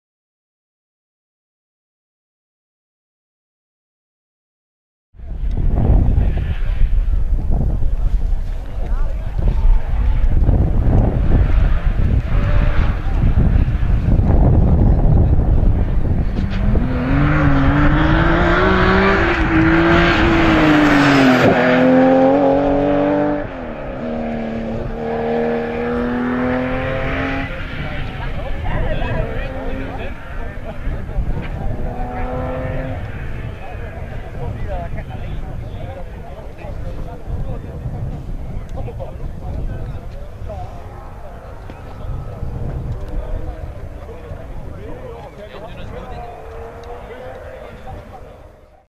Porsche 911 fire in the ass !!!!
Janssens lors du TAC rally sur sa porsche 911 qui crache de superbes flamme